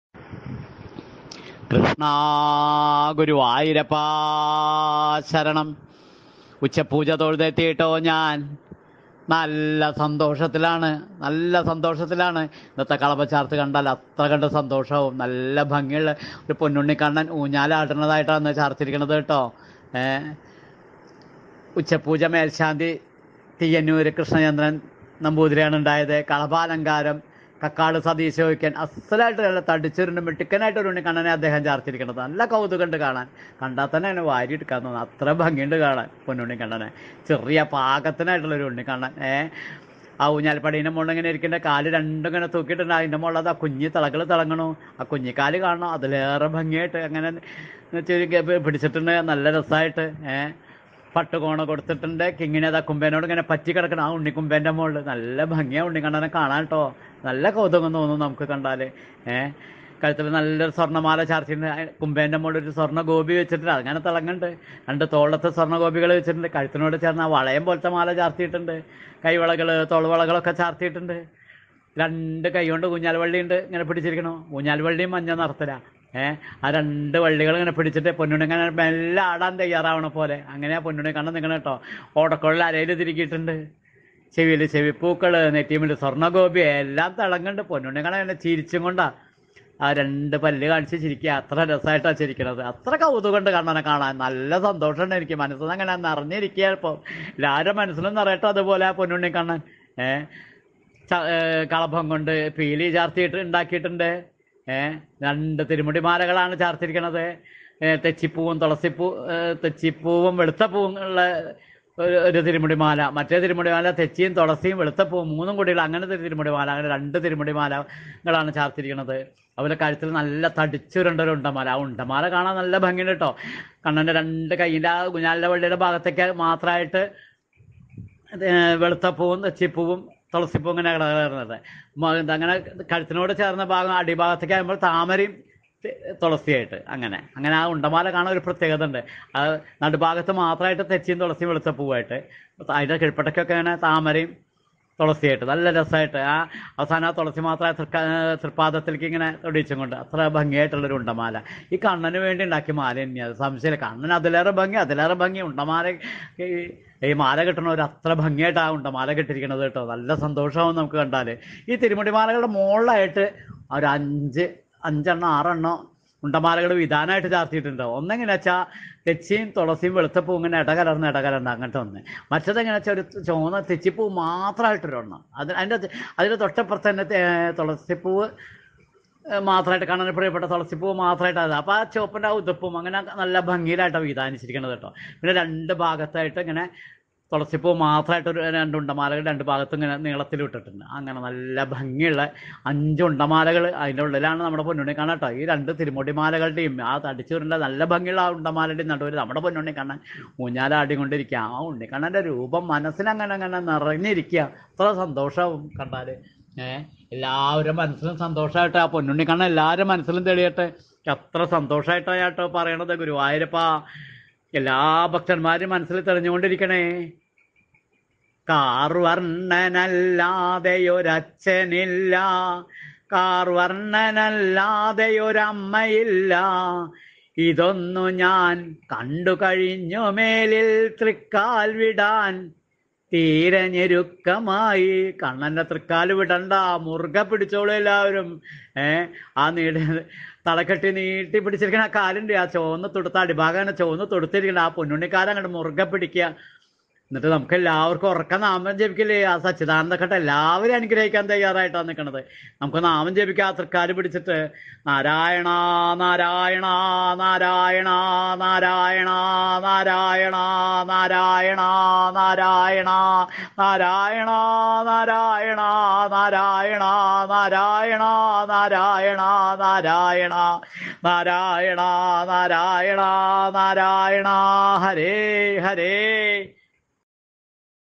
ഗുരുവായൂരപ്പൻ്റെ ഉച്ചപൂജ അലങ്കാരവർണ്ണന
🕉 Sree Krishna Guruvayurappa Saranam 🙏🏼🙏🏼🙏🏼🙏🏼🙏🏼. Today’s detailed description of the Uchapuja Alankaram of our Sree Krishnan @ Guruvayur 🙏🏼🙏🏼🙏🏼🙏🏼🙏🏼